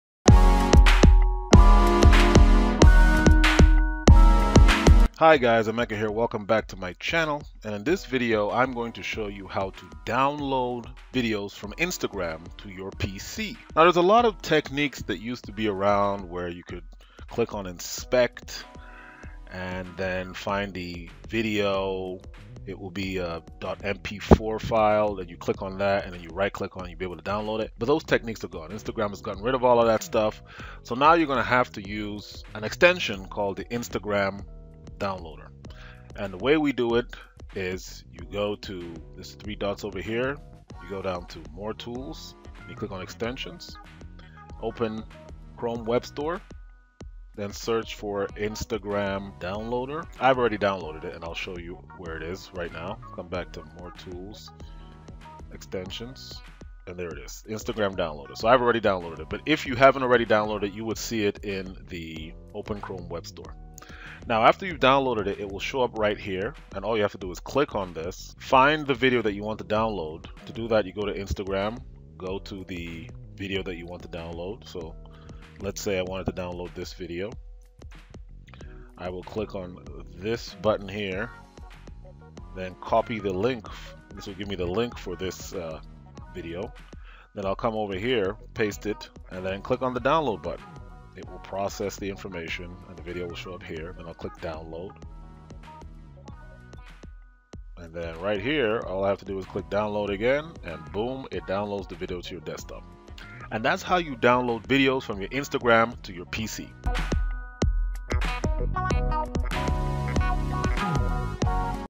How to download videos from Instagram to your PC
In this video I will show you how to download Instagram videos to your computer.